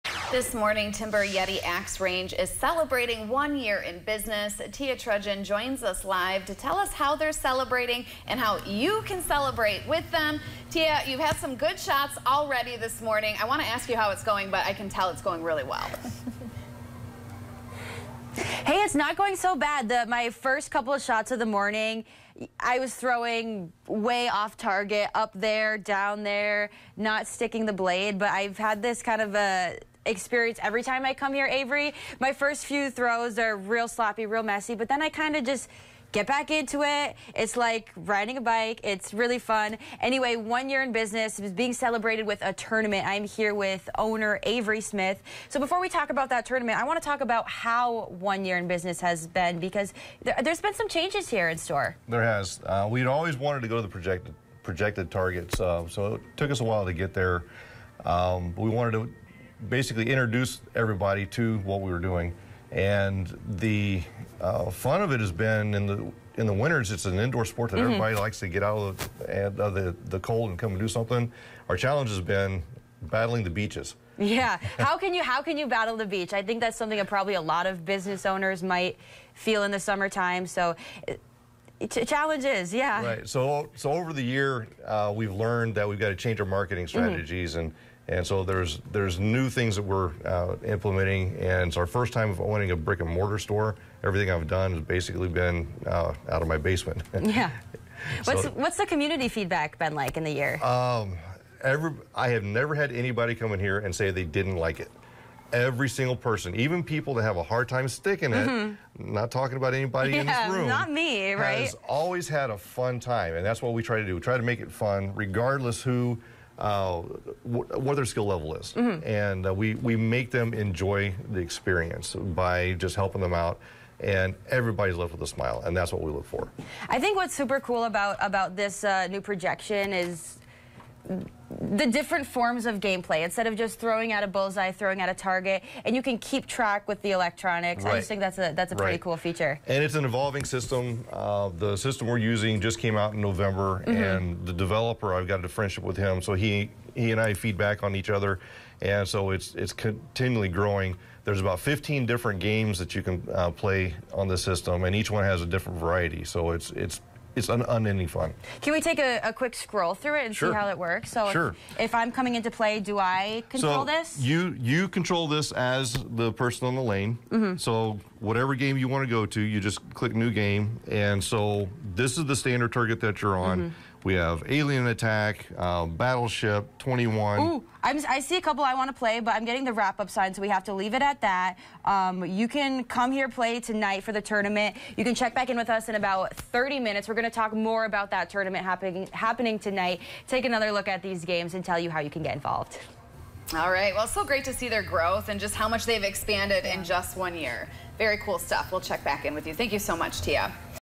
Timber Yeti Axe Range celebrates one year anniversary
TV6's Tia Trudgeon chats with Avery Smith about what's new at Timber Yeti and how the business is celebrating one year.